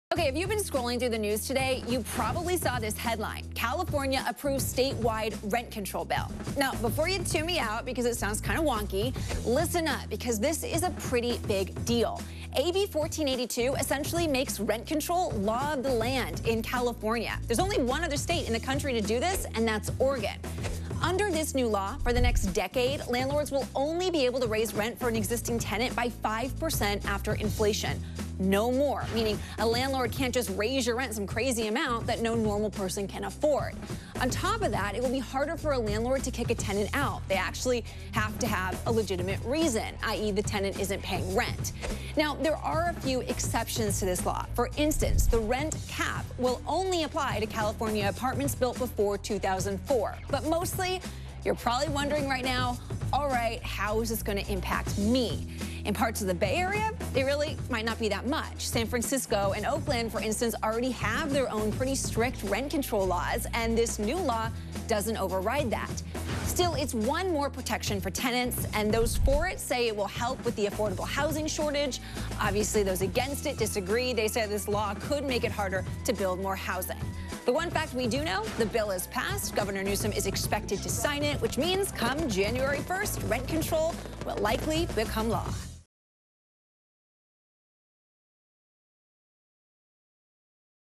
Breaking down California's new rent control bill
The rent cap bill, AB1482, essentially makes rent control the law of the land in California. There's only one other state in the country do this and that's Oregon.

-- Follow us for breaking news as it happens.